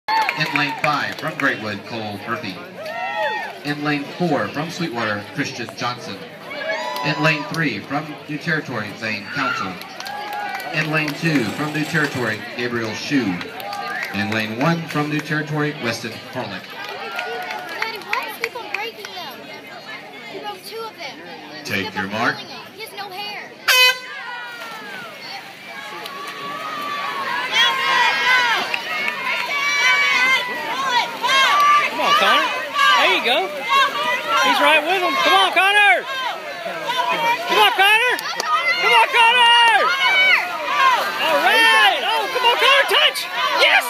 Connor Butterfly stroke final 2012
Connor Winning Butterfly stroke in exciting fashion